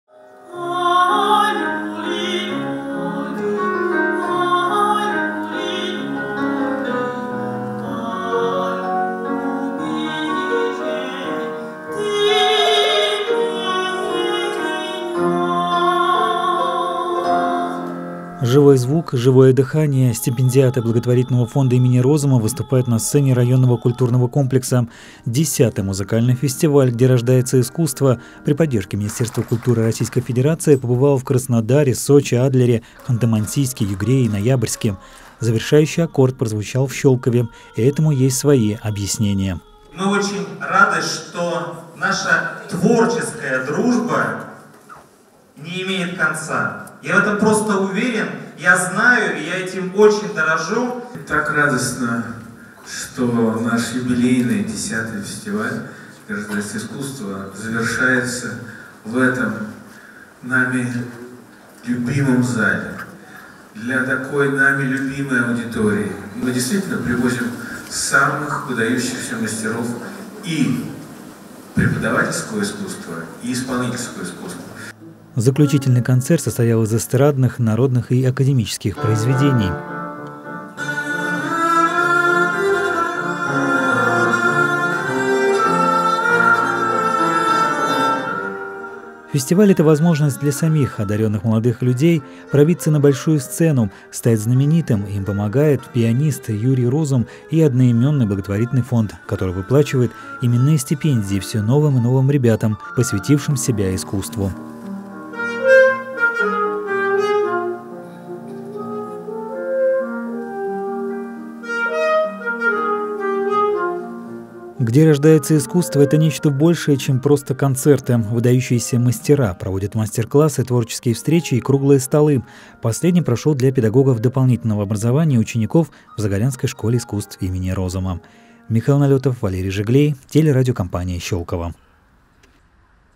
Закрытие фестиваля «Где рождается искусство»
Где рождается искусство. Закрытие музыкального фестиваля с участием народного артиста Юрия Розума прошел в Районном культурном комплексе. Преуспевающие в творчестве молодые дарования из школ искусств и музыкальных школ показали, что знают намного больше основного курса обучения.